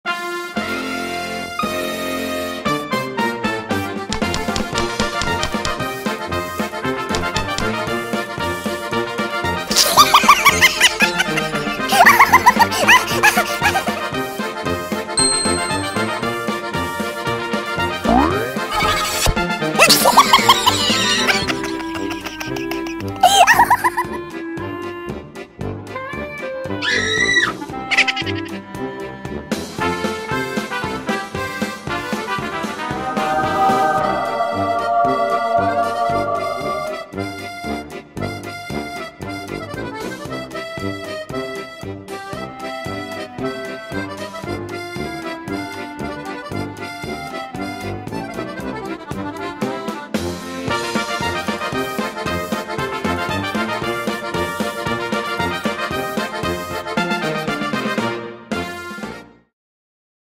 What is mishka up to?
What is mishka up to? In this series there will be a funny bear, a fat SpongeBob, and Patrick, who was afraid of a rat